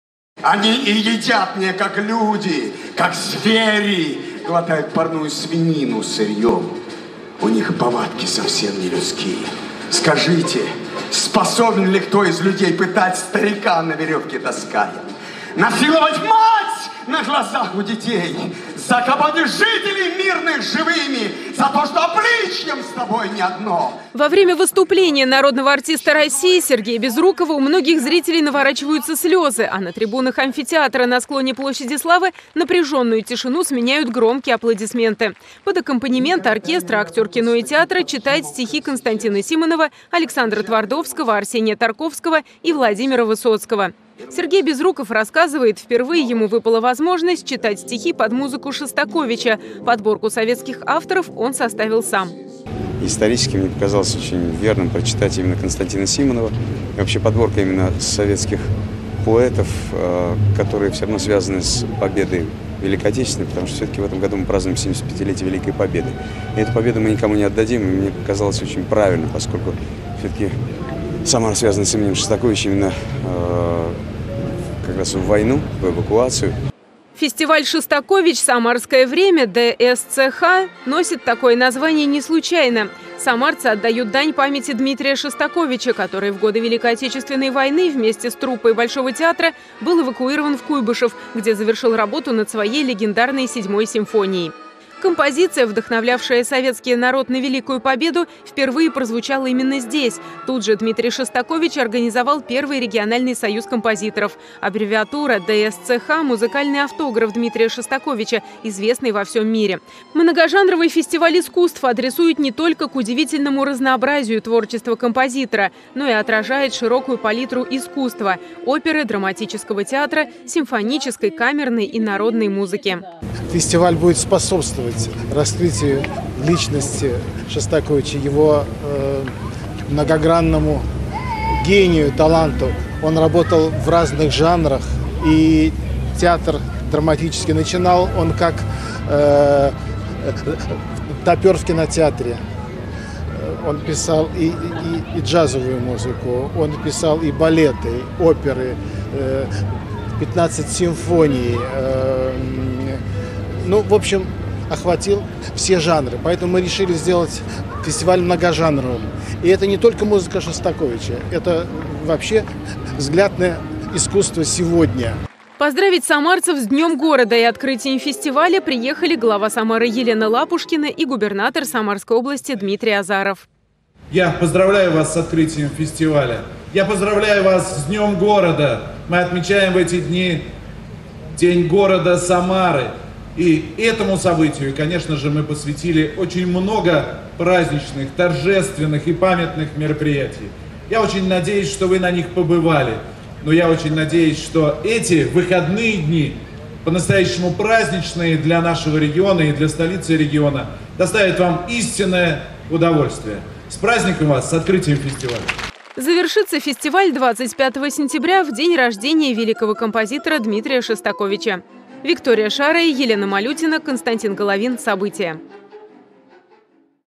В Самаре на склоне площади Славы стартовал Международный фестиваль искусств "Шостакович"
В Самаре на склоне площади Славы стартовал Международный фестиваль искусств "Шостакович. Самарское время DSCH" при поддержке Министерства Культуры и администрации города посвященный 434-му Дню рождения города. Сред приглашенных выдающиеся артисты и музыканты. Подробнее в сюжете.